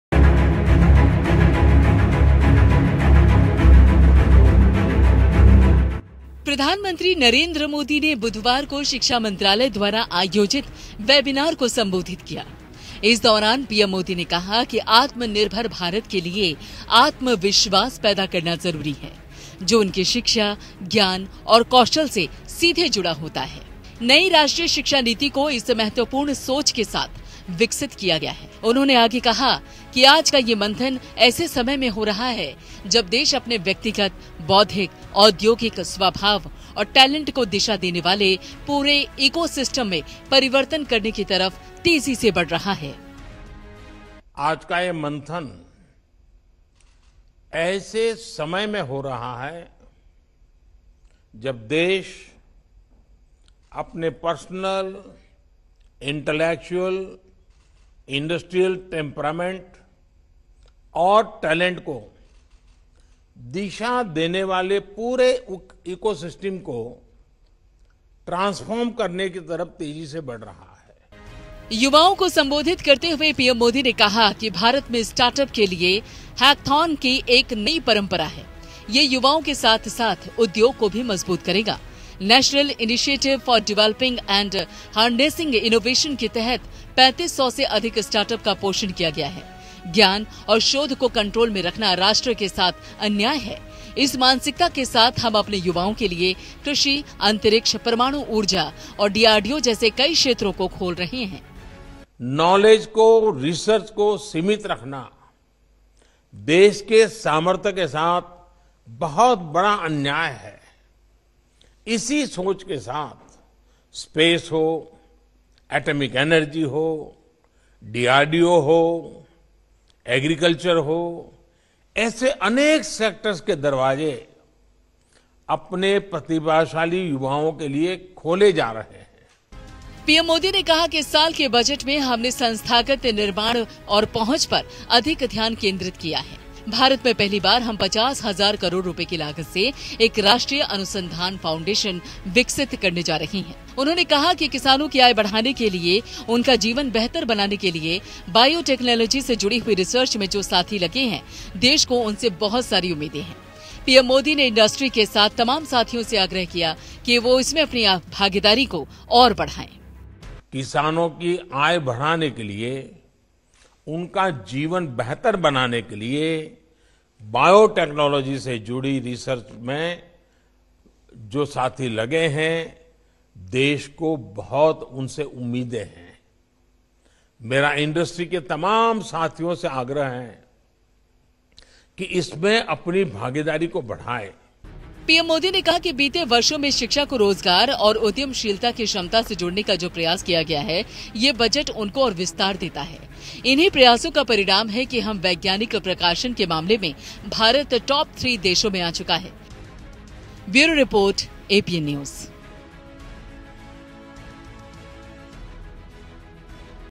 PM Modi ने Webinar को किया सम्बोधित, कहा- AatmaNirbhar Bharat के लिए आत्मविश्वास जरूरी !
PM MODI ने शिक्षा मंत्रालय द्वारा आयोजित वेबिनार को संबोधित किया. इस दौरान पीएम मोदी ने कहा कि आत्मनिर्भर भारत के लिए आत्मविश्वास पैदा करना जरूरी है. जो उनकी शिक्षा, ज्ञान और कौशल से सीधे जुड़ा होता है. नई राष्ट्रीय शिक्षा नीति को इस महत्वपूर्ण सोच के साथ विकसित किया गया है.' उन्होंने आगे कहा, 'आज का ये मंथन ऐसे समय में हो रहा है, जब देश अपने व्यक्तिगत, बौद्धिक, औद्योगिक स्वभाव और टैलेंट को दिशा देने वाले पूरे इकोसिस्टम में परिवर्तन करने की तरफ तेजी से बढ़ रहा है.'